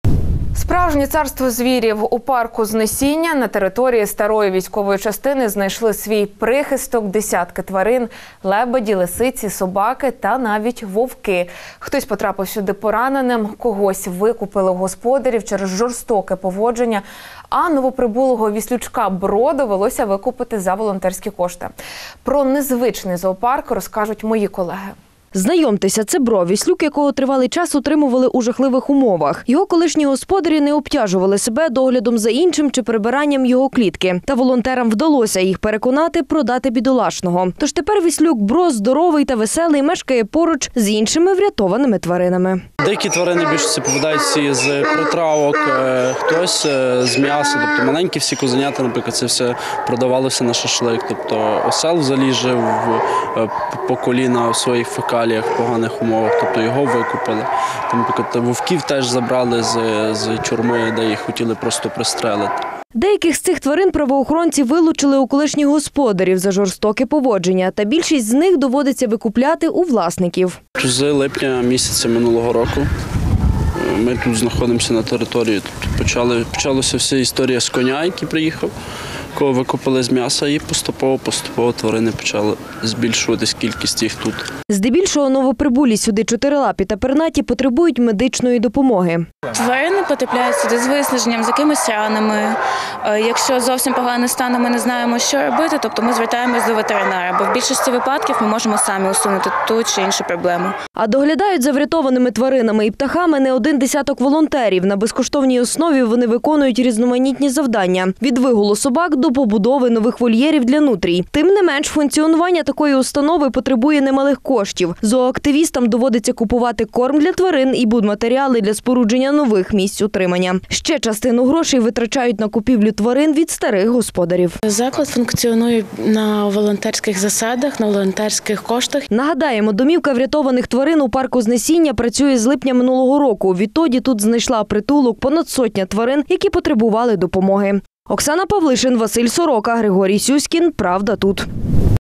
Життя врятованих тварин. ПравдаТУТ Львів
Справжнє царство звірів. У парку Знесіння, на території старої військової частини, знайшли свій прихисток десятки тварин. Лебеді, лисиці, собаки та, навіть, вовки - хтось потрапив сюди пораненим, когось викупили у господарів через жорстоке поводження, а новоприбулого віслючка Бро довелося викупляти за волонтерські кошти.